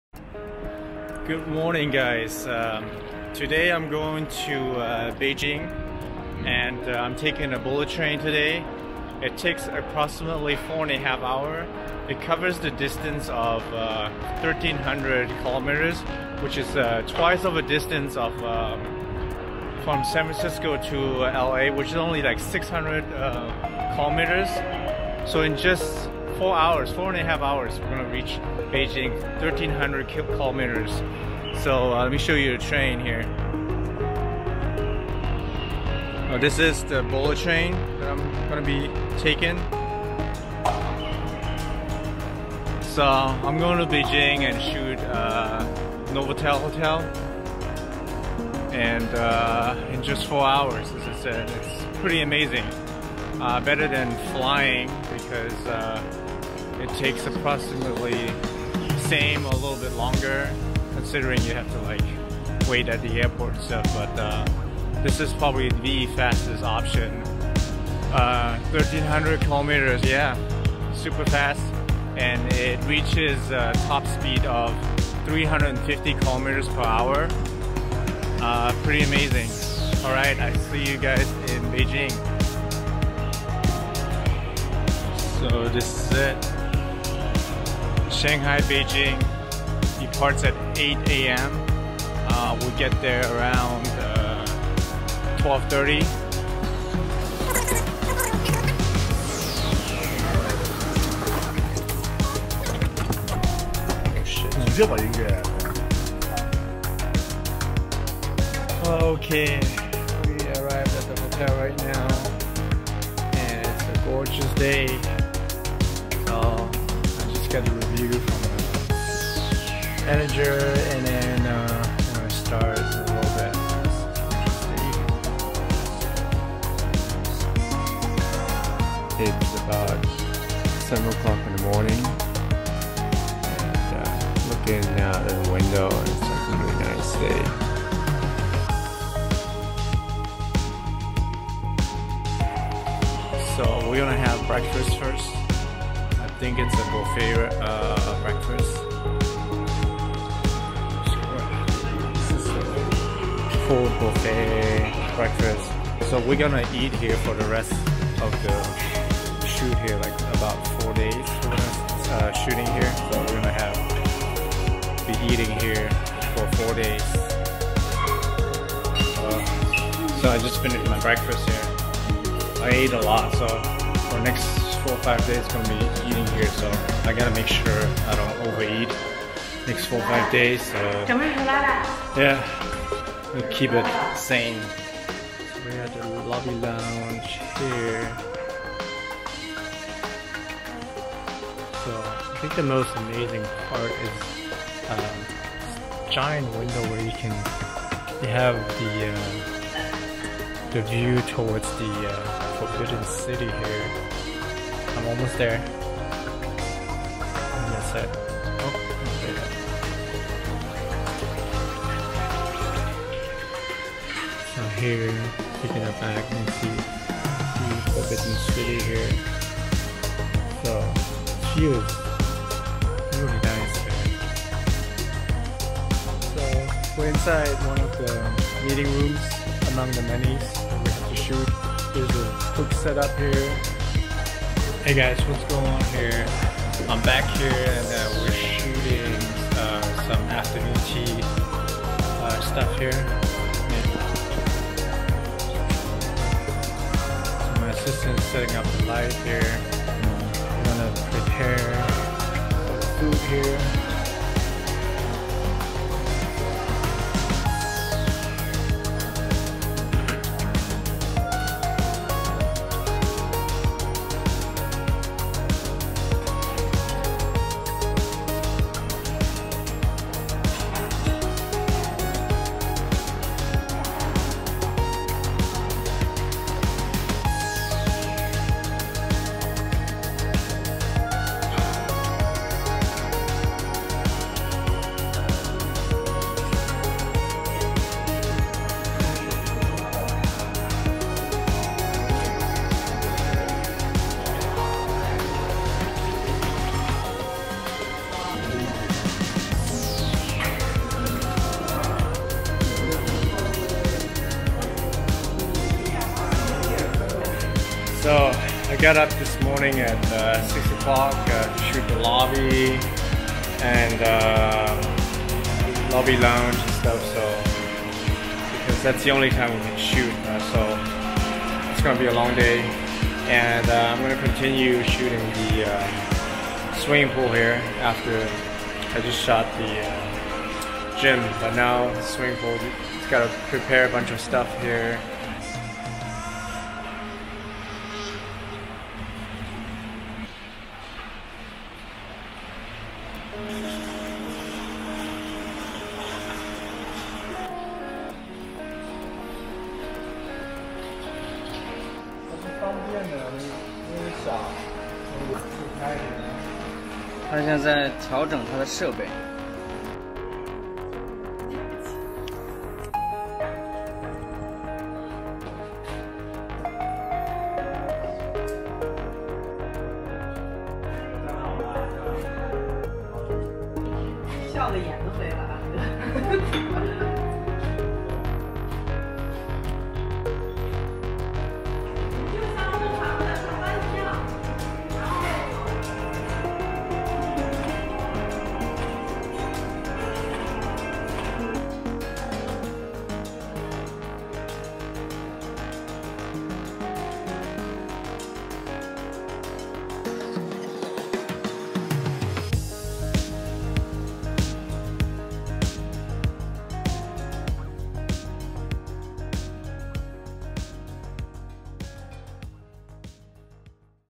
04 VLOG: Novotel Beijing Photo Shoot Fastest Bullet Train
Taking the fastest train from Shanghai to Beijing (1300 KM/ 800  Miles) in just 4.5h. That's twice the distance of Los Angeles - San Franciso (400 miles). Some behind the scenes for our shoot at the Novotel in Beijing.